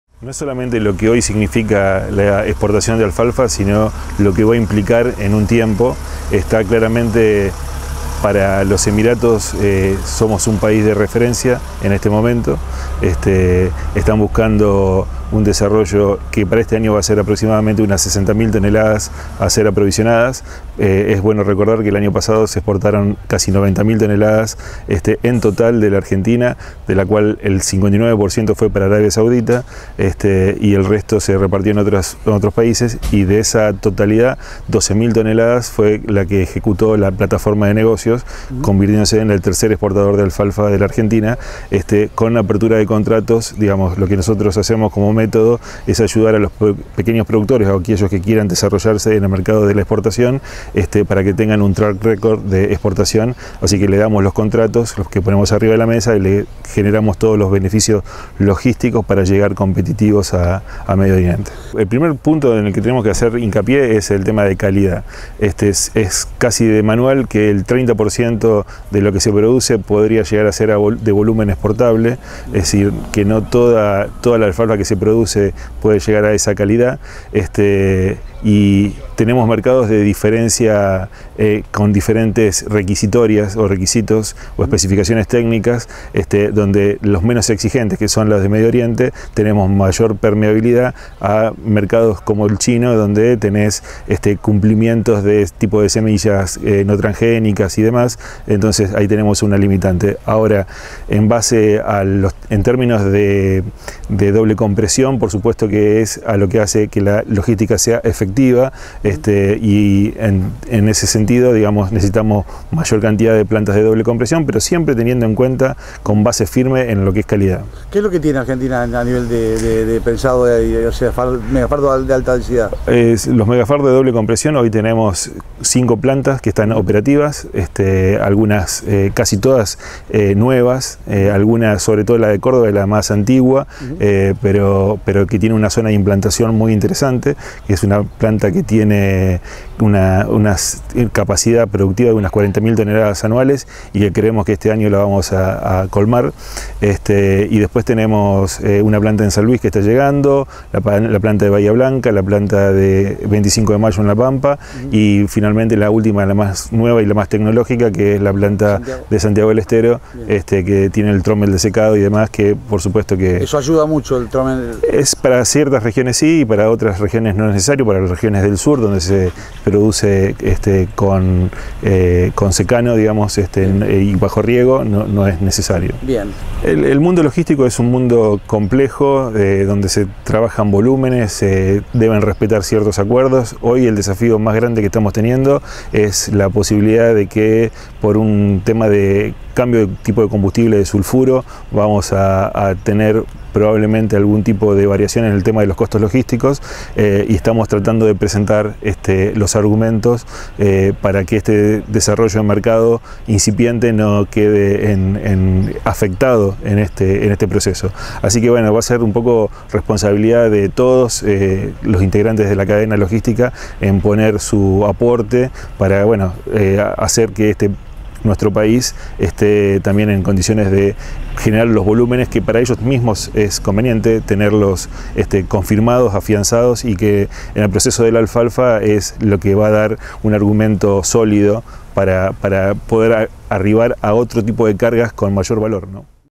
¿Cómo está la oferta y demanda mundial de productos base alfalfa?
En el marco de las Jornadas Nacionales de Alfalfa 2019 dialogamos con Gabriel Osatinsky, presidente de la Cámara Emiratí Argentina.